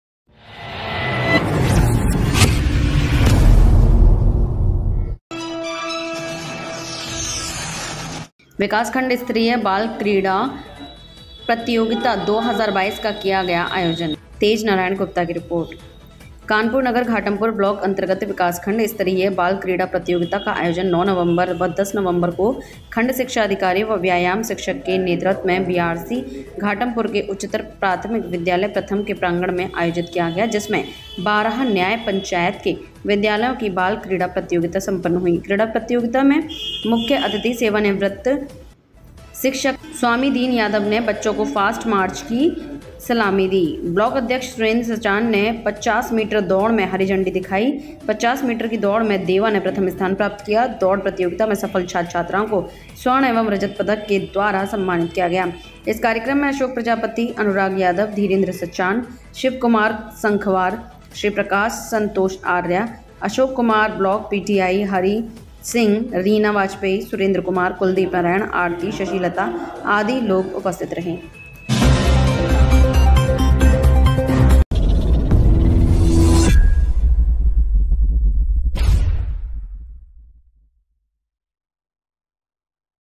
विकासखंड स्तरीय बाल क्रीड़ा प्रतियोगिता 2022 का किया गया आयोजन
ब्लॉक रिपोर्टर -तेज नारायण गुप्ता।                                       कानपुर नगर घाटमपुर ब्लॉक-अंतर्गत विकासखंड स्तरीय बाल कीड़ा प्रतियोगिता का आयोजन 09 नंबर व 10 नवंबर को खंड शिक्षा अधिकारी व व्यायाम शिक्षक के नेतृत्व में बीआरसी घाटमपुर के उच्चतर प्राथमिक विद्यालय प्रथम के प्रांगण में आयोजित किया गया। जिसमें बारह न्याय पंचायत के विद्यालयों की बाल क्रीड़ा प्रतियोगिता संपन्न हुई। क्रीडा प्रतियोगिता में मुख्य अतिथि सेवानिवृत्त  शिक्षक श्री स्वामी दिन यादव जी ने बच्चों की फास्ट मार्च की सलामी ली। ब्लॉक अध्यक्ष सुसेंद्र सचान ने 50 मीटर दौड़ में हरी झंडी दिखाई 50 मीटर की दौड़ में देवा ने प्रथम स्थान प्राप्त किया। दौड़ प्रतियोगिता में सफल छात्र छात्राओं को स्वर्ण एवं रजत पुरस्कार के द्वारा सम्मानित किया गया।इस कार्यक्रम में अशोक प्रजापति, अनुराग यादव, धीरेंद्र सचान, शिवकुमार संखवार, श्रीप्रकाश, संतोष आर्या, अशोक कुमार, ब्लॉक पी0टी0आई हरि सिंह, रीना बाजपेई, सुरेंद्र कुमार, कुलदीप नारायण, आरती, शशि लता, प्रभा प्रजापति, ज्योत्स्ना, प्रियंका आदि अनुदेशक कार्यक्रम में उपस्थित रहे।